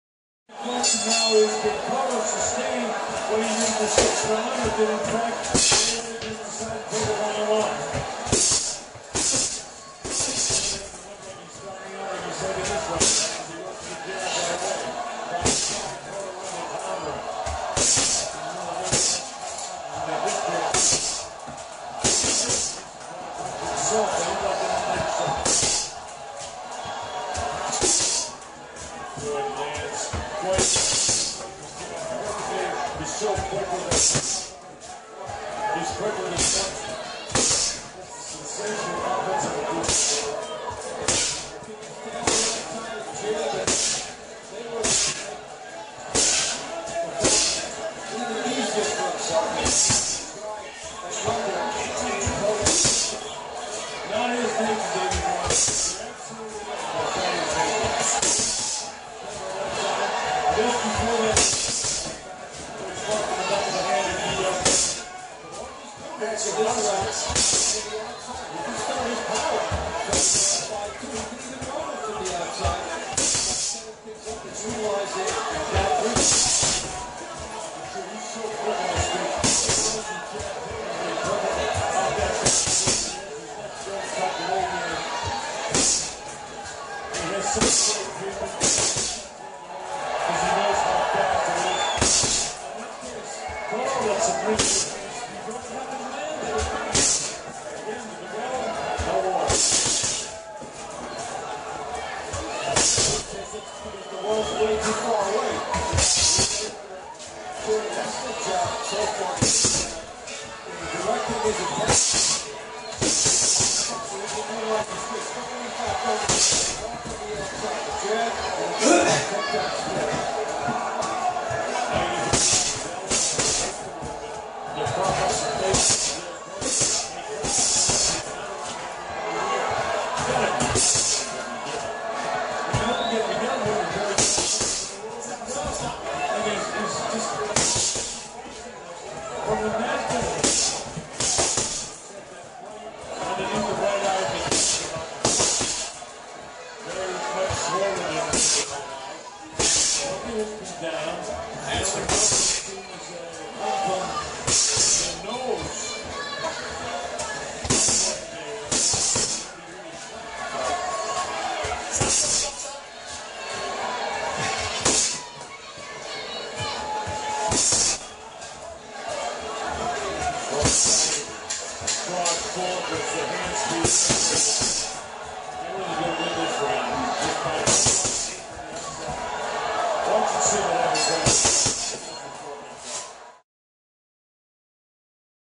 Pretty solid Heavy Bag workout. :c)
I felt really good about that round, especially from 30sec to 2  minutes. That was pretty strong. Always been a fan of my "Maidana" inspired Right hook to the body/left hook to the body /overhand right but in the last few training I definitely stepped it up with jab/cross/ left hook to the body /overhand right .